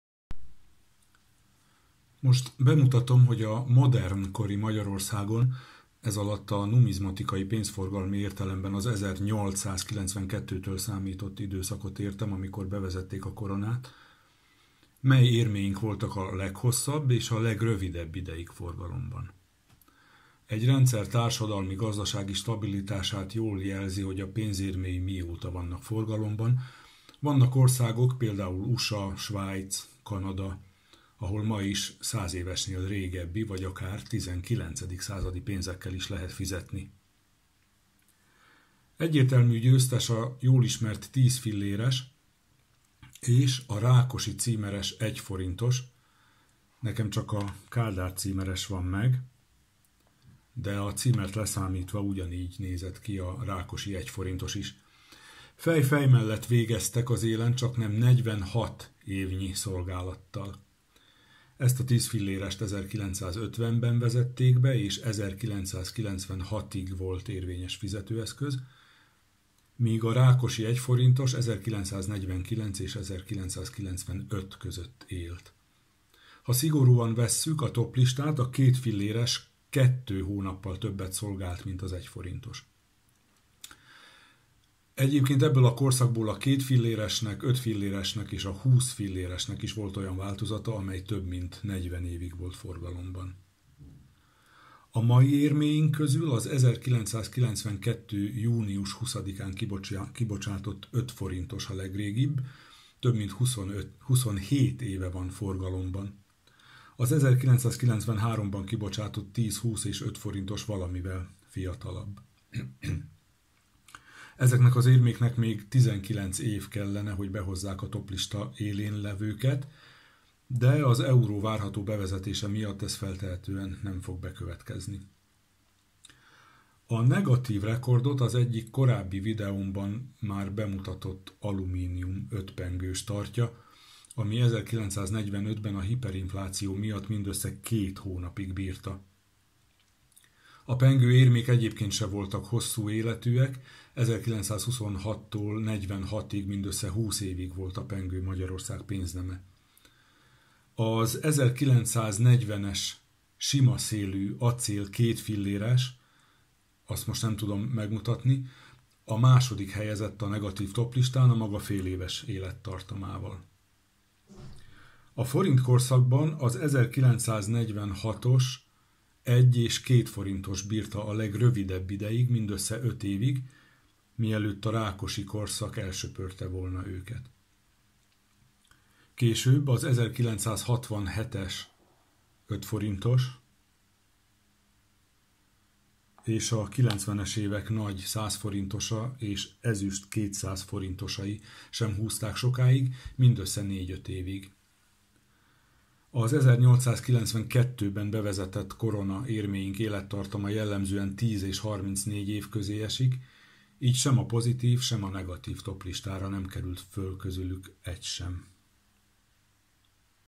Melyik érménk volt legtovább forgalomban?
A leghosszabb ideig, csaknem 46 évig a jól ismert 10-filléres volt forgalomban, míg az 1945-ös 5-pengős élettartama mindössze két hónap volt.